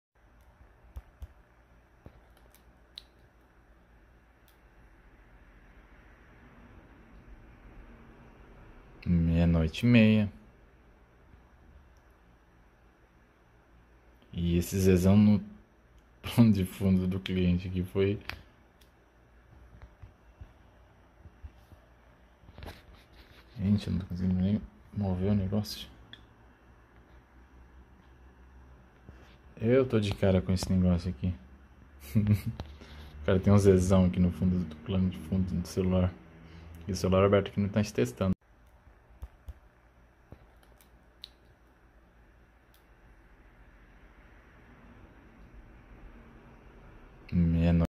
Nunca vou tankar o plano de fundo desse cliente, MUITO BOM!
Fui trocar a tela do cliente e ... quando eu vi, não me aguentei rsrs
@CanalZezao nós te amamos, e meu cliente é seu paga pau hahaha